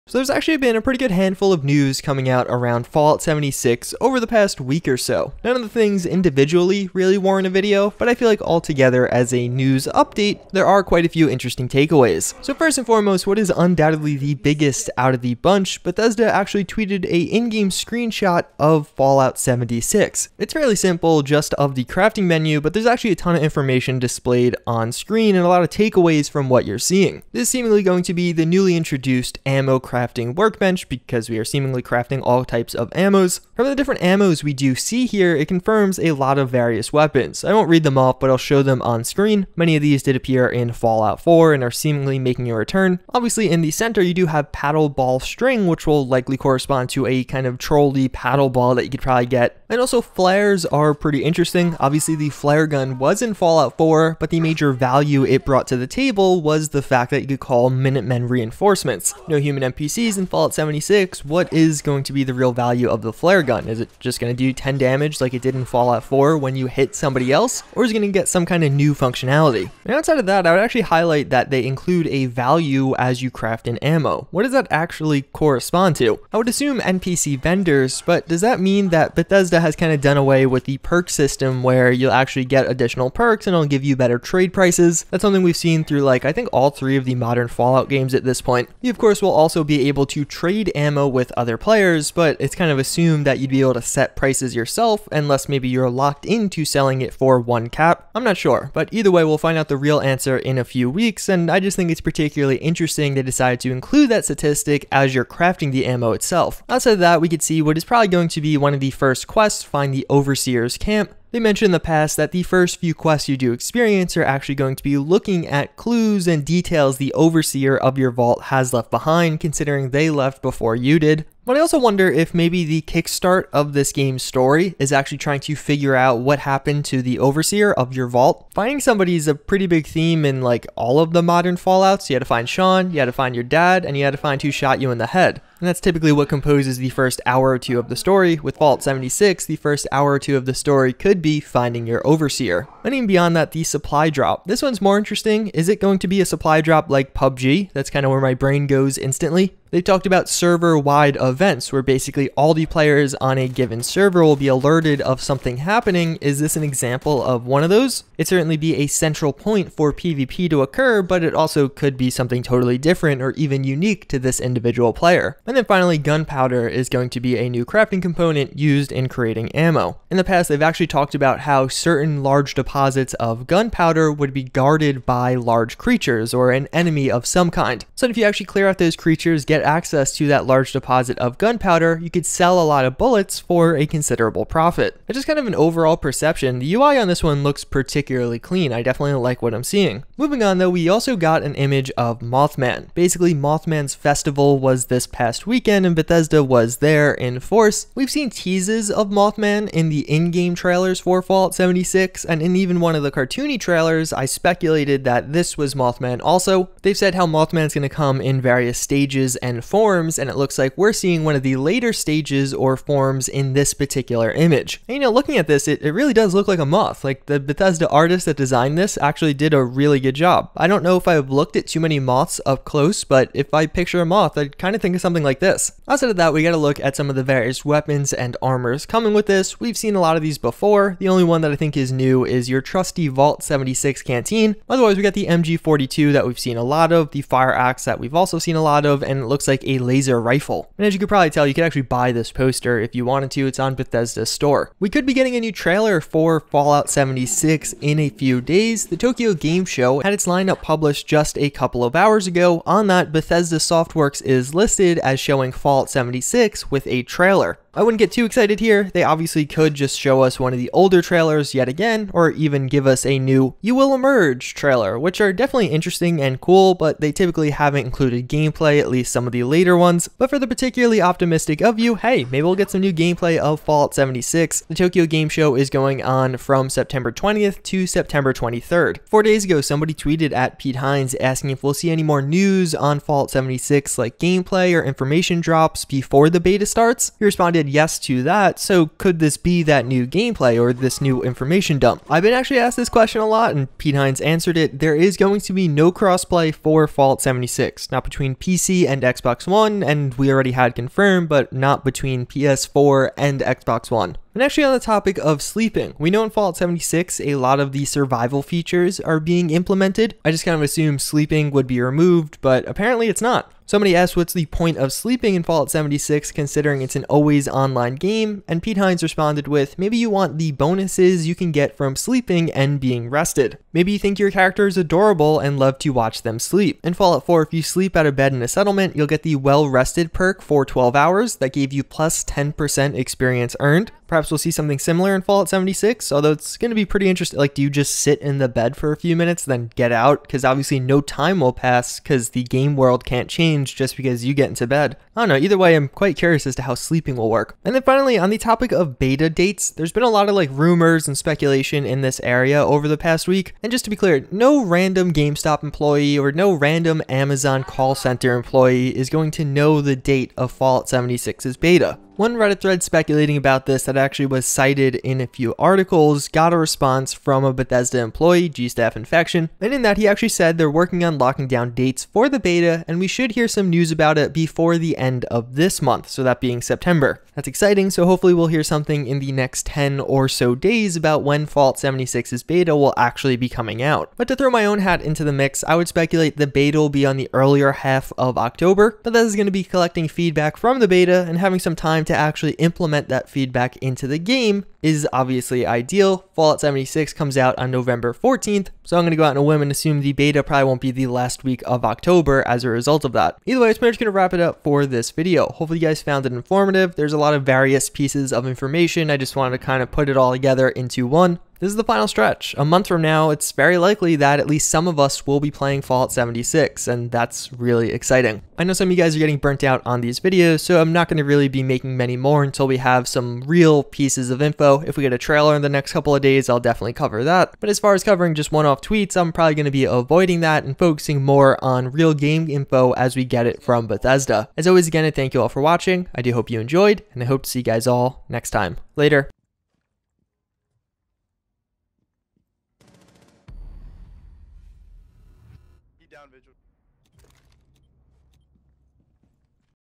Fallout 76 New Info - Next Trailer, New In Game Screenshot, BETA Date Info Soon
Today we take a look at some new details around Fallout 76, this one in particular looks at Fallout 76's BETA, a new screenshto that was released, and even a potentially new Fallout 76 trailer.